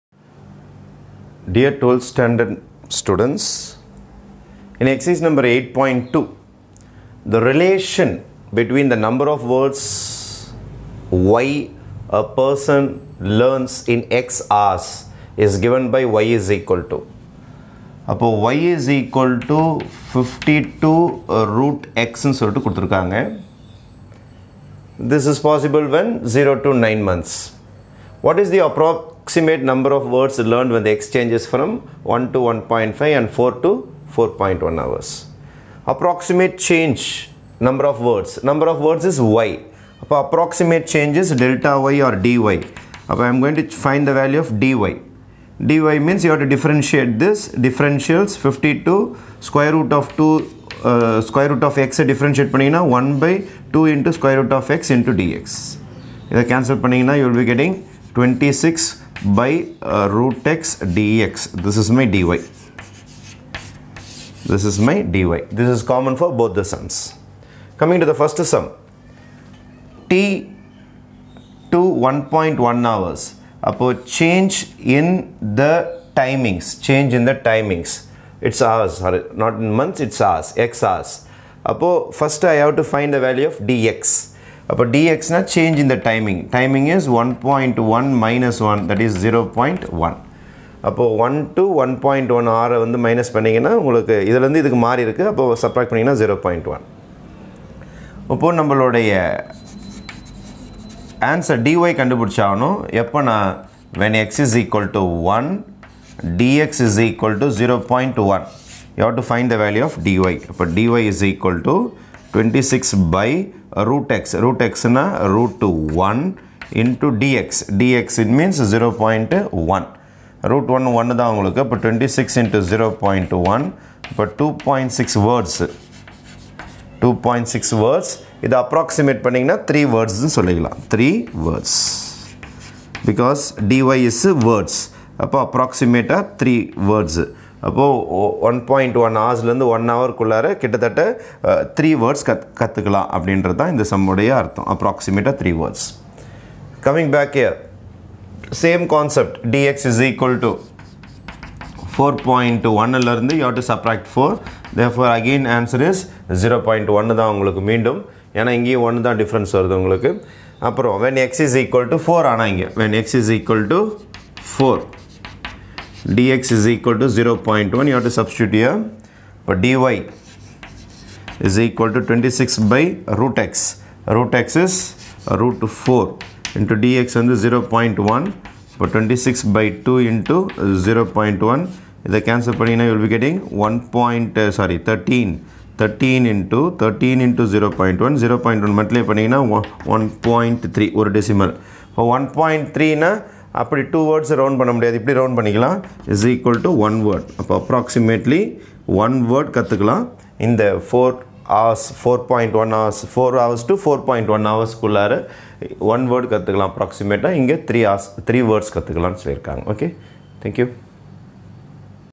12th Std Maths Ex.8.2(9) The relation between the number of words y a person learns in x hours is gi
12th Std Maths

CHAPTER 8 DIFFERENTIALS AND PARTIAL DERIVATIVES